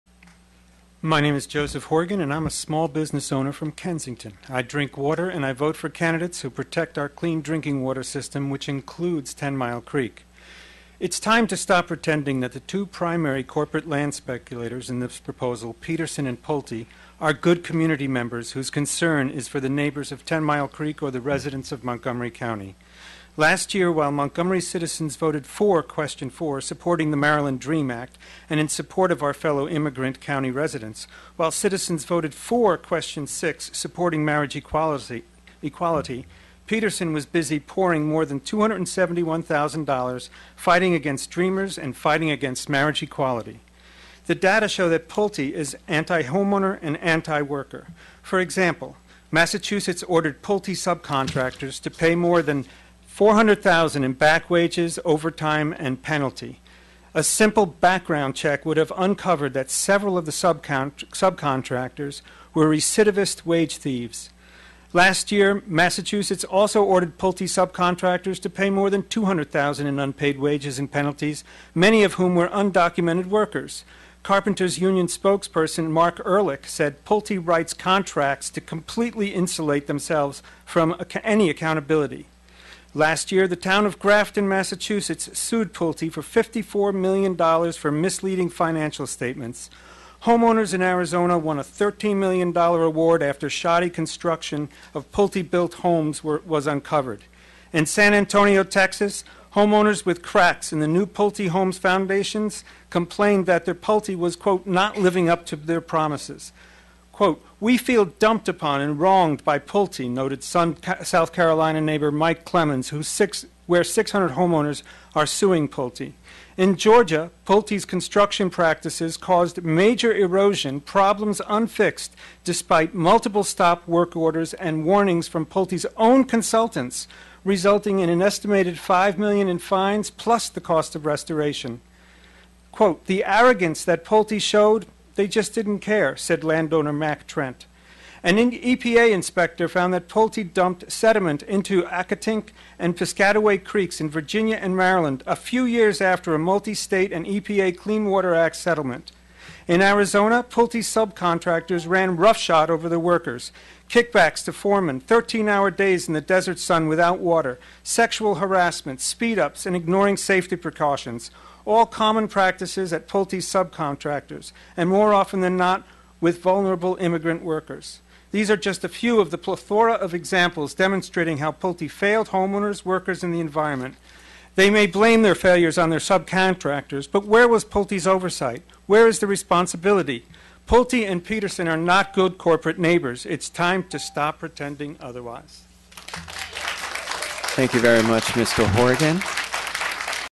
Joseph Horgan Drinks Water
Montgomery County Council Hearings from Dec. 5th, 2013 on development in the Ten Mile Creek watershed - source of emergency drinking water for 4.3 million residents in the Washington DC metropolitan area.